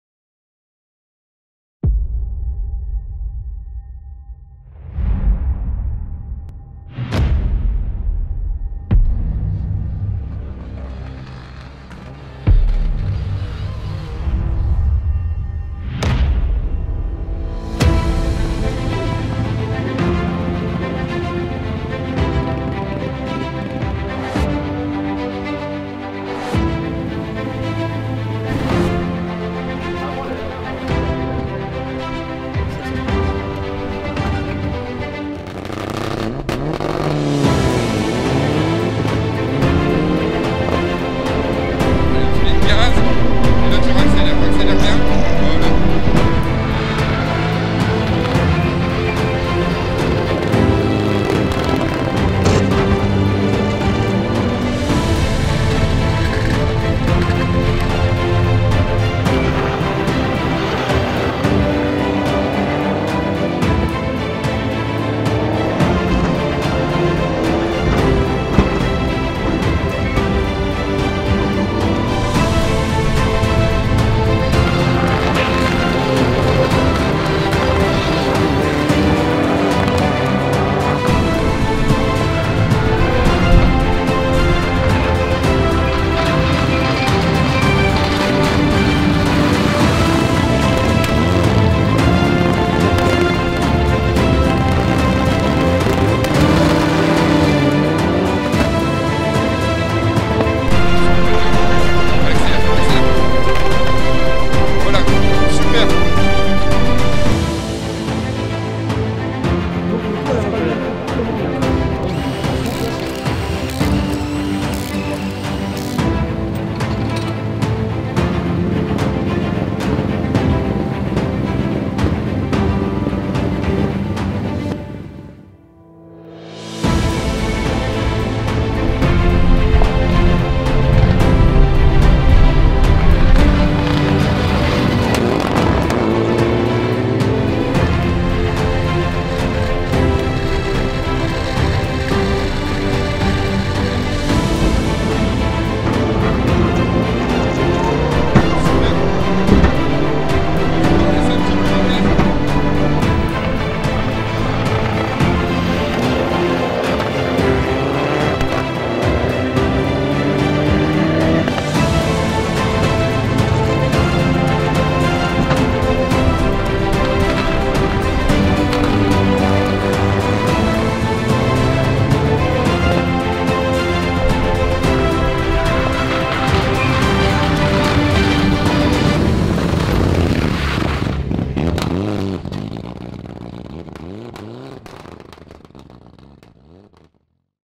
Test Day Rallye / Valery Macron
En ce début d'année particulier Valery Macron à eu l'occasion de faire quelques kilomètres d'essai à bord de sa Mitsubishi flambant neuve 🤩
L'occasion parfaite pour ressortir les caméras en attendant des jours meilleurs ! 😞

🎥 RealShot